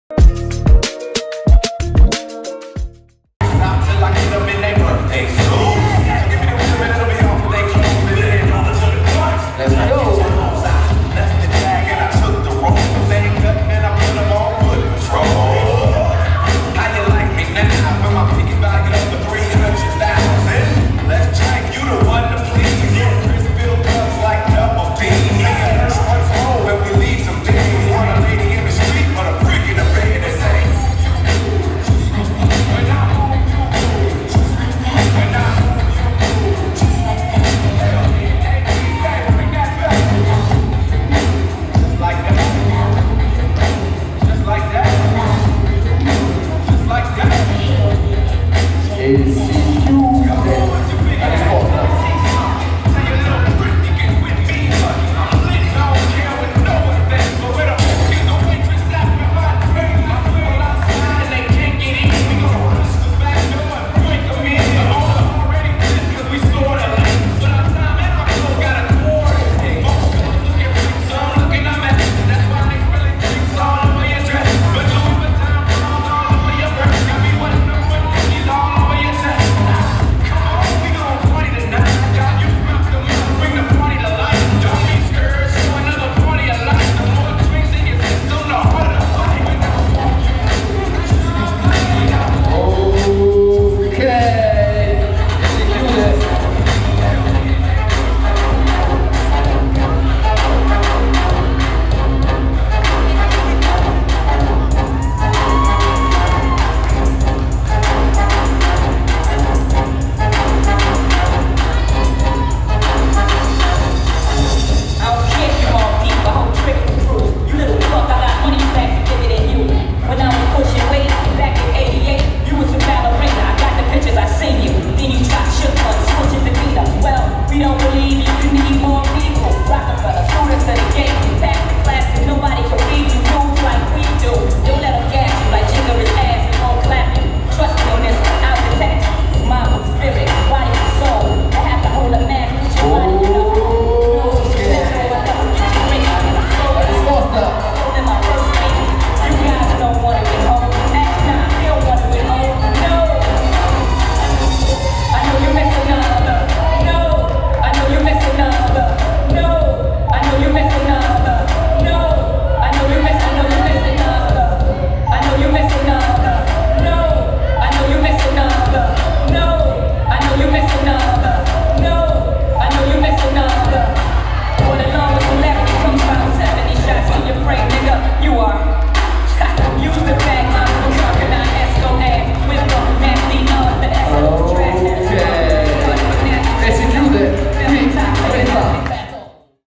Nik & Gio vs Mads & Soul [4] Hip-Hop Over 14 | King Of Warriors 2019
King Of Warriors by Street Warriors Crew (12/05/2019)
Hip-Hop Contest

Quarti di finale categoria Student Over 14

Winner: Mads & Soul